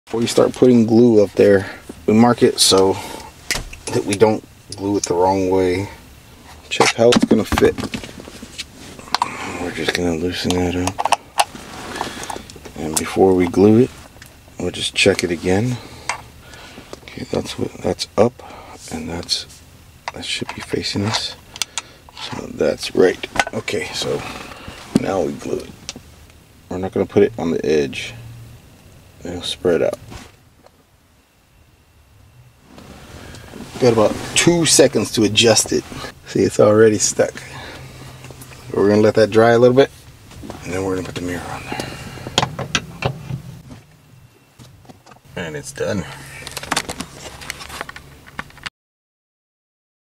rearview mirror install
I made this video to highlight that sometimes even "small jobs" require specialty tools. Investing in your tools will help SAVE MONEY in the long run.